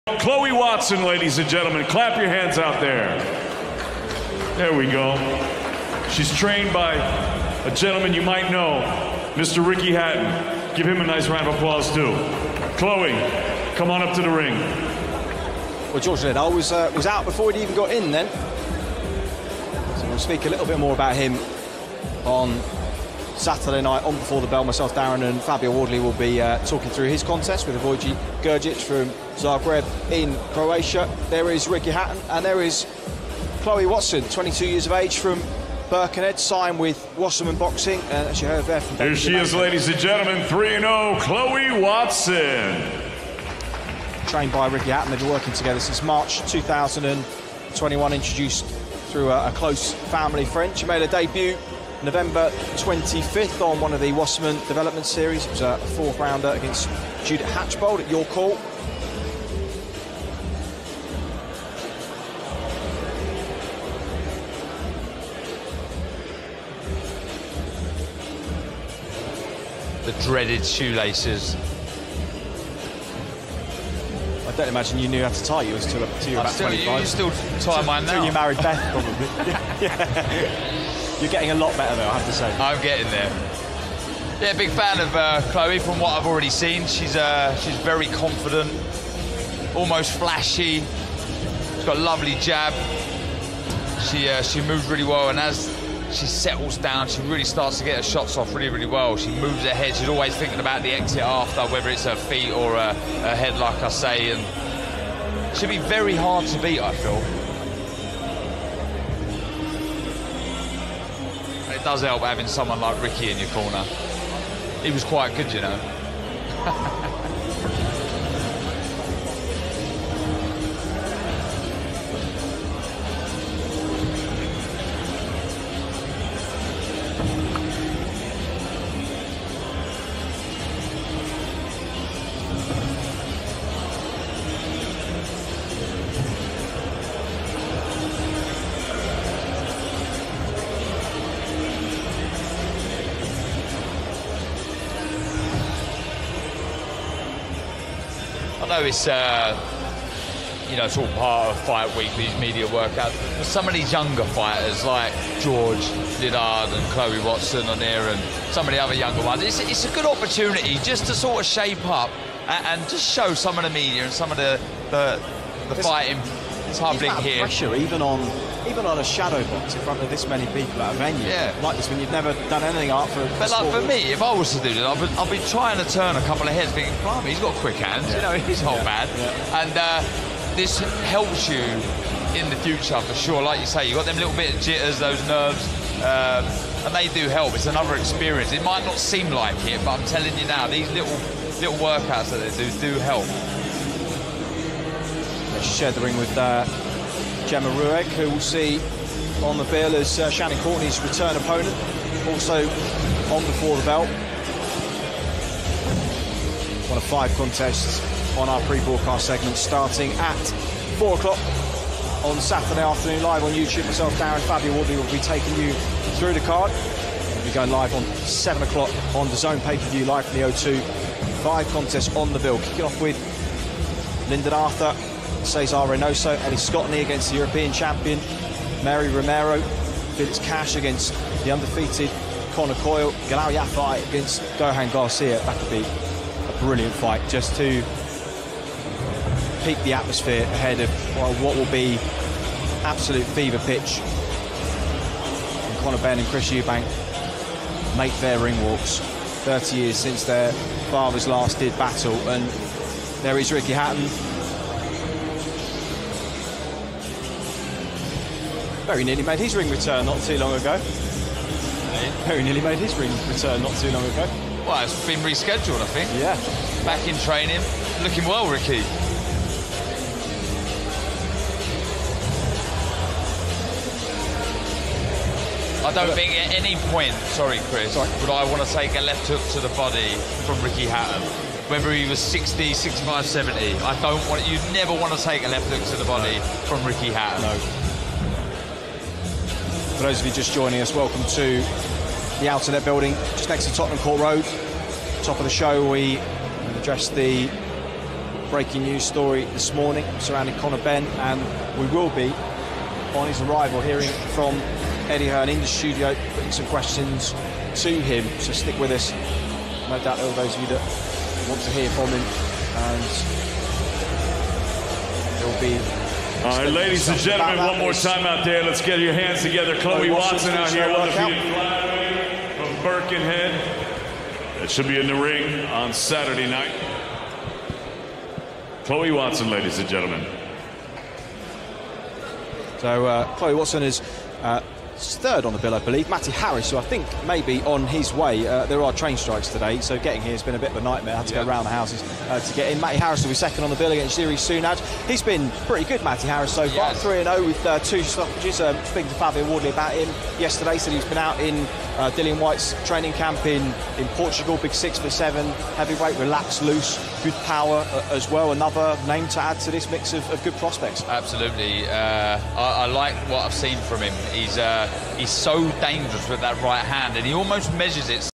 Chloe Watson | Public workout | Eubank Jr v Benn Undercard
Todays public workout for Chloe Watson at the start of a mad week in London. Accompanied by Ricky Hatton.